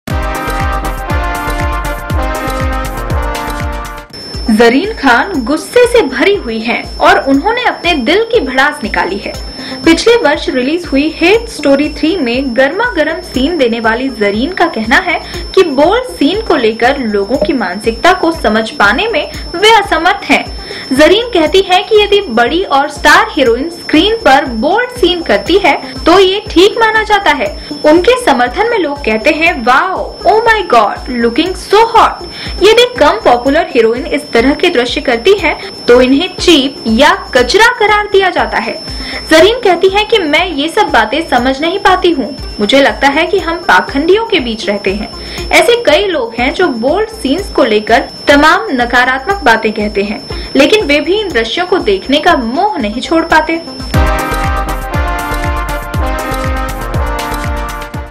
Bold Scenes... वे करें तो वाह-वाह... हम करें तो 'चीप' : खान
ज़रीन खान गुस्से से भरी हुई हैं और उन्होंने अपने दिल की भड़ास निकाली है। पिछले वर्ष रिलीज हुई 'हेट स्टोरी 3' में गरमा-गरम सीन देने वाली ज़रीन का कहना है कि बोल्ड सीन को लेकर लोगों की मानसिकता को समझ पाने में वे असमर्थ हैं।